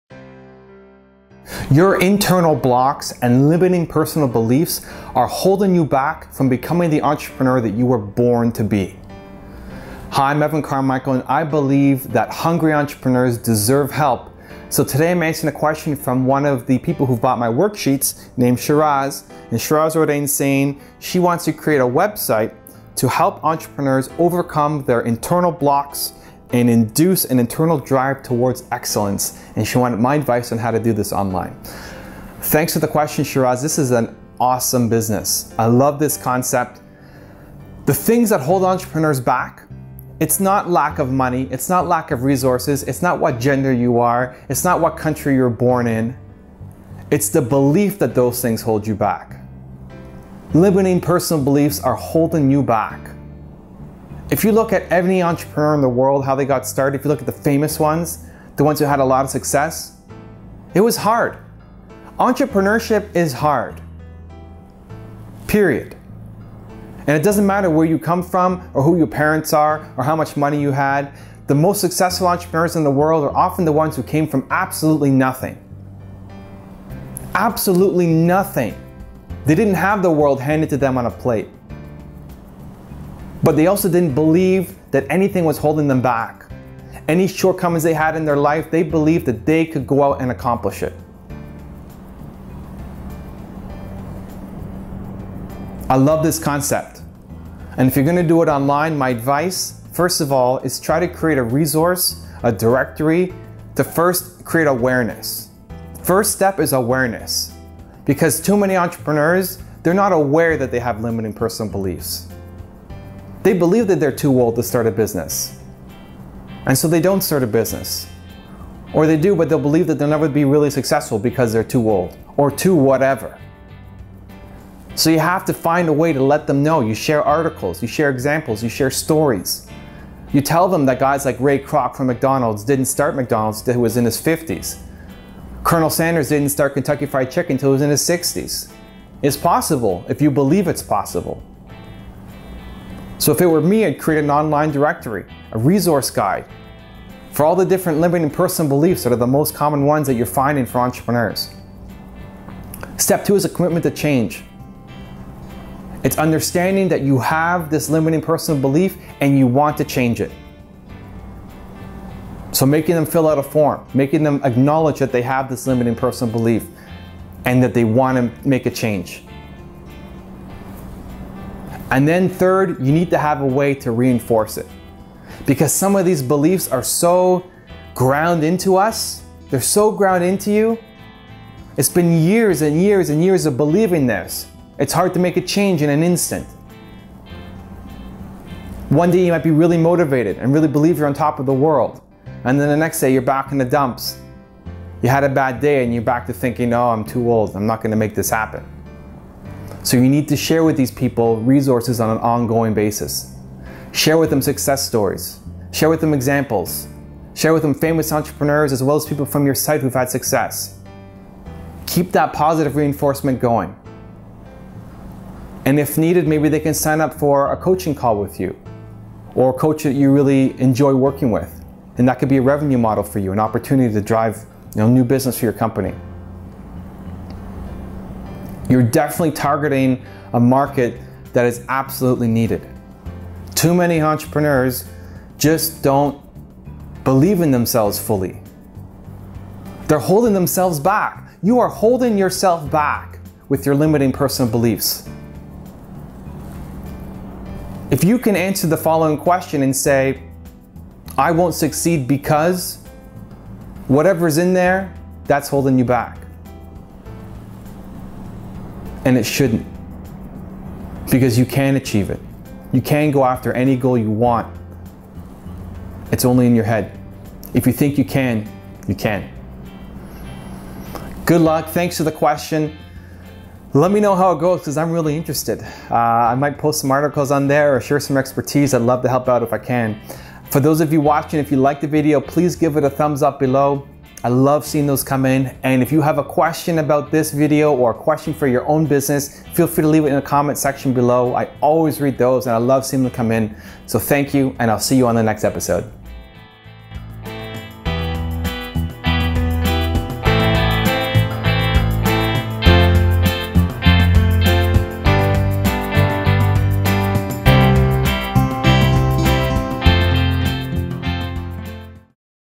Your internal blocks are holding you back!
In this video I answer a question from one my readers who asked:

"Hi Evan

Thanks for the interest.  I find the worksheets to be great.  It has a good balance between being informative and the advice articles also make them inspirational.  I have a passion and value combination that adds up to the full 20 marks.

I know this mail is a bit long so if you feel like dumping it, I will understand.  If you manage to read through it though, I will appreciate your feedback.

Very often the challenges faced by the entrepreneur goes beyond the "skill and knowledge" facet of business.  It penetrates right into the psyche of the person and makes all the knowledge and systems redundant because the will and courage to act is reduced to waste because of internal blocks or belief systems that is rooted in the sub-conscious.

Sometimes it can even be physical limitations.  When my daughter was six years old and started school in grade 1, she was diagnosed with a brain disorder that caused hand-eye coordination problems.  My wife and I were devastated when told that we should not expect much from her academically because her reading and writing abilities would be compromised as a result.  I started to develop interventions that would change her brain dynamics to that of an empowered learner and she developed into an A- average Engineering Drawing student and is currently one of the top interior and architectural design students in her faculty at college, and she needs no external motivation to charge towards her destiny.

Below is an extract from an email that I received this morning from a young lady in her early twenties requesting my help.  This is a pervasive problem with our youth as well as adults.  My system can take someone like this from despair to an empowered individual and they will continue onto a path of realization with their drive from inside.  I removed all personal info from the extract."